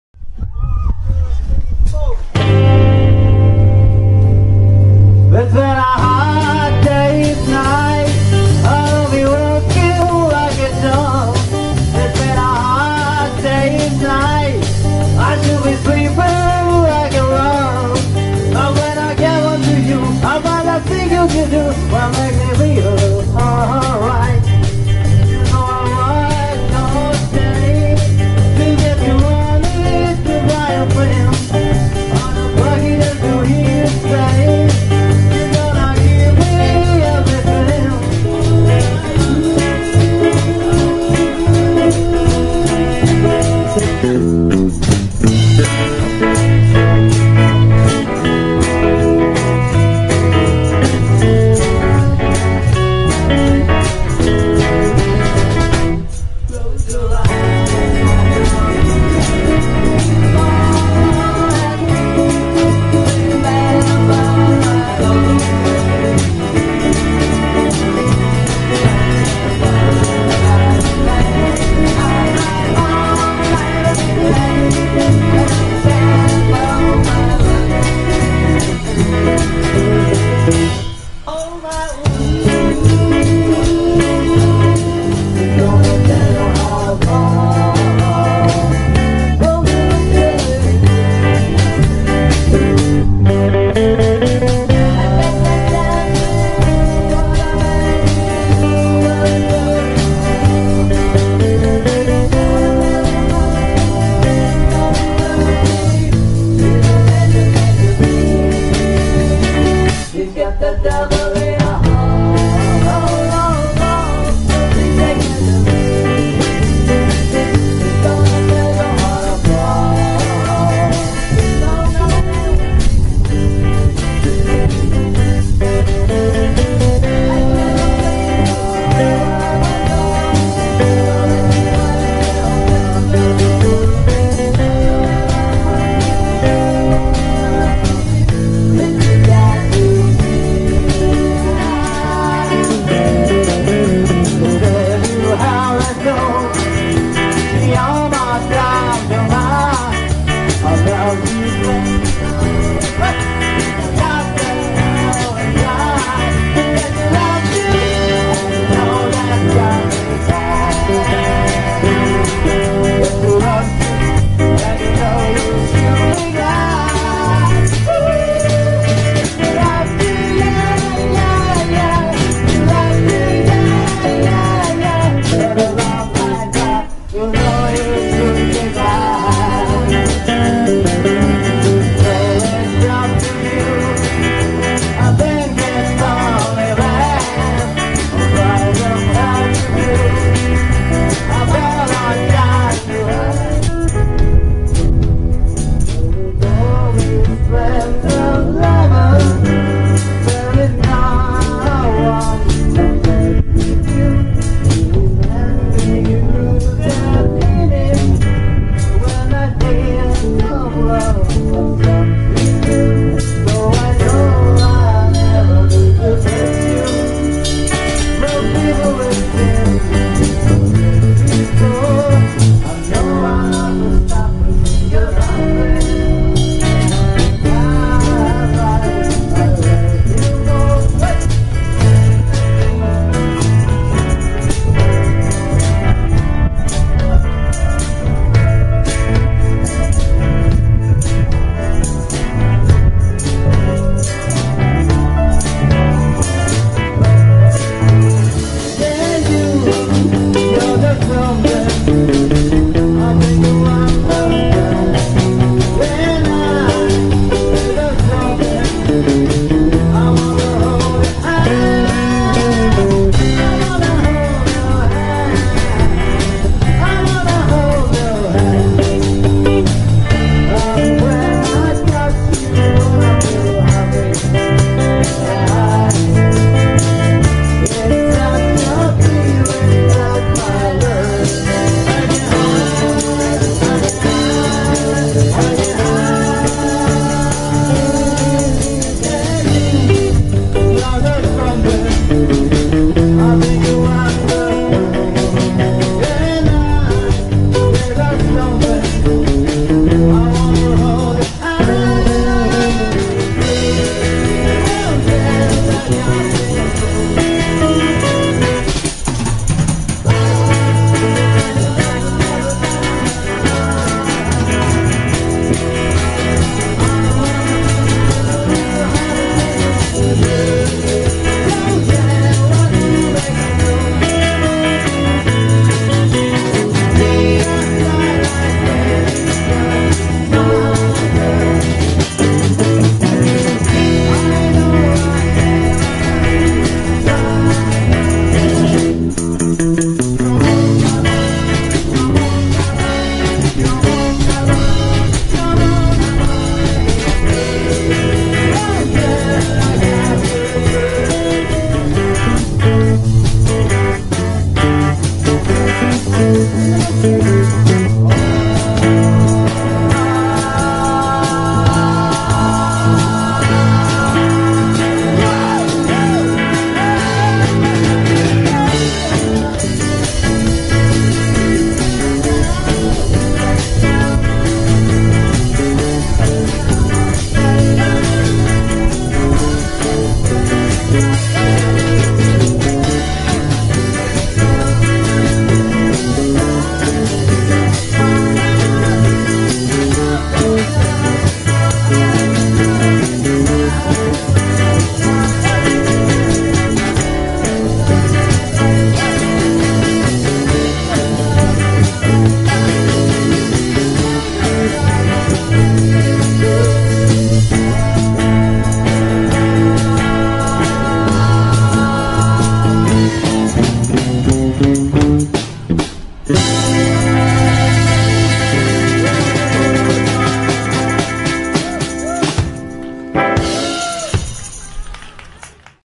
「ビートルズ・トリビュート・バンド・ライブ」その６
急なイベント営業でしたけど、諭吉さん複数枚のギャラ＆ディナー付きというありがたいお仕事でした💦